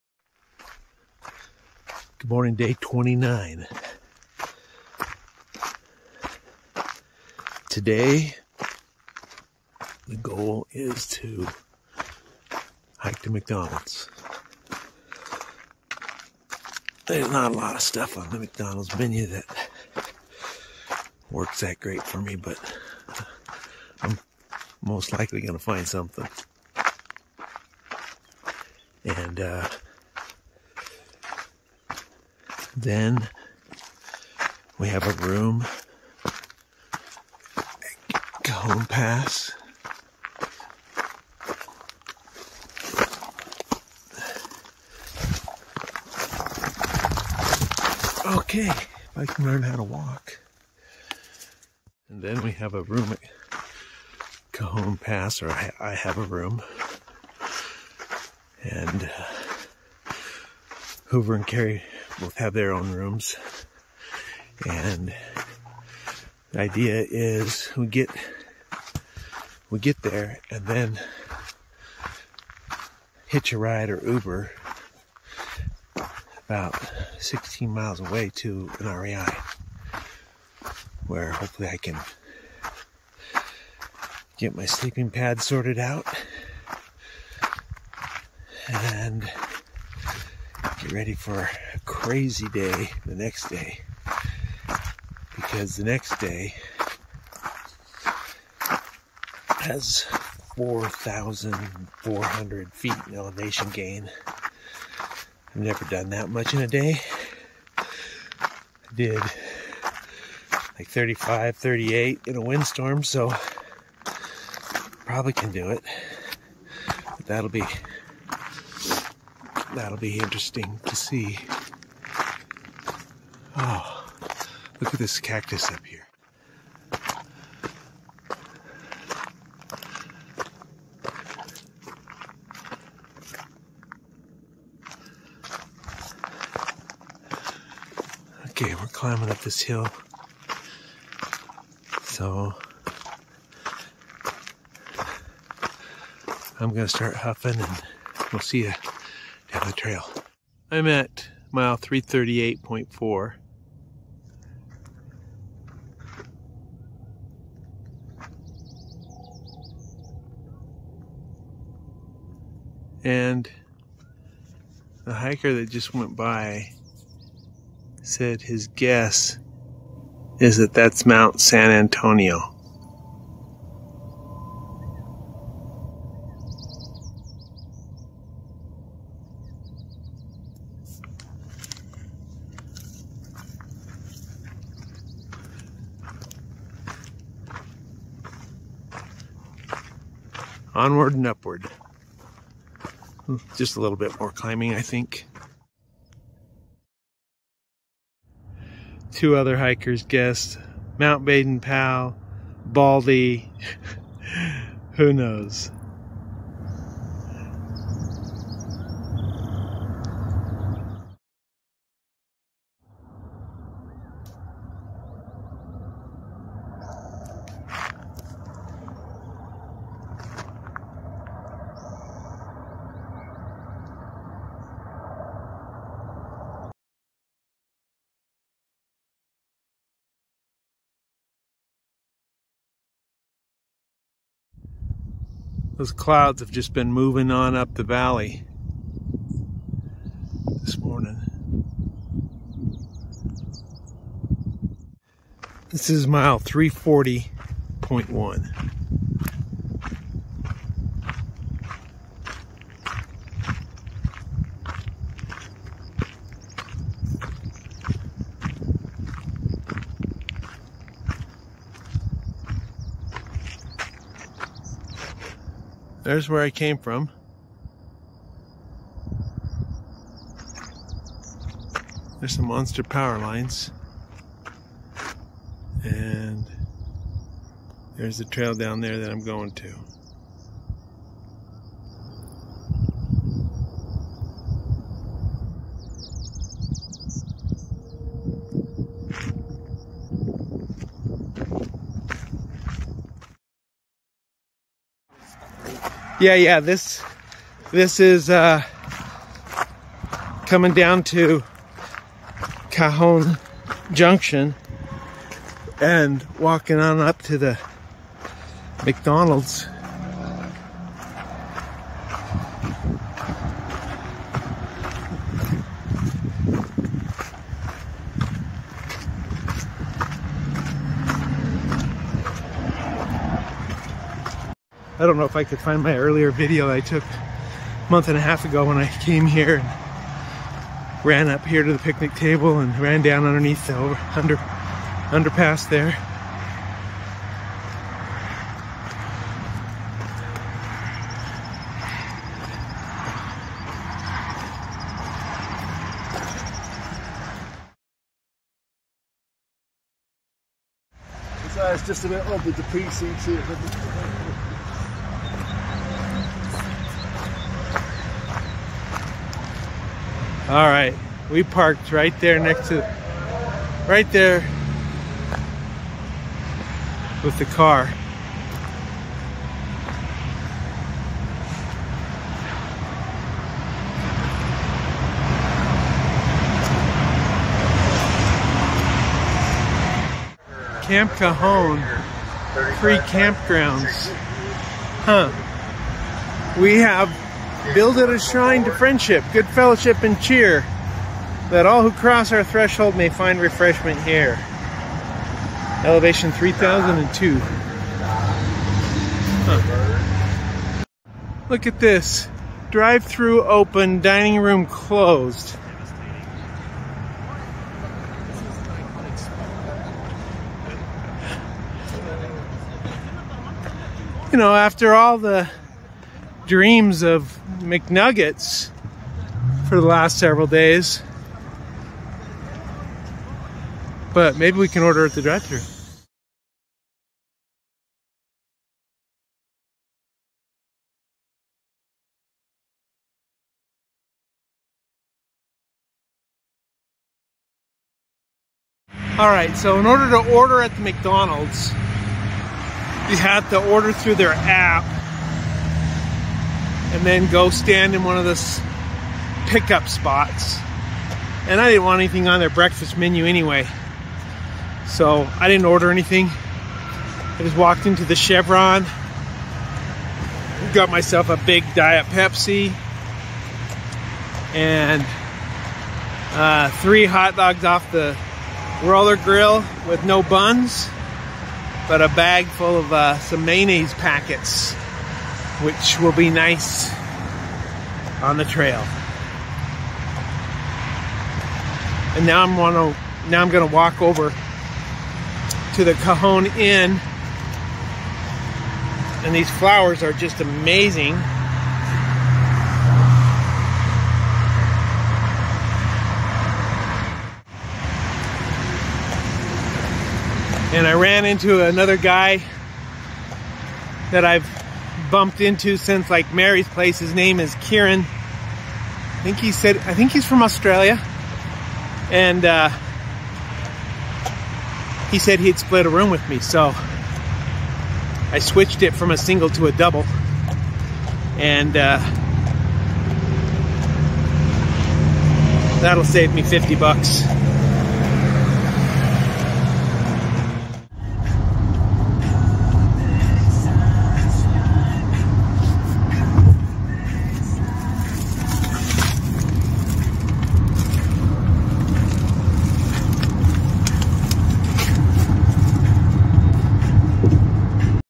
Day 29 - Diabadass on the PCT 2022
Nero Hiking into Cajon Pass and going to REI.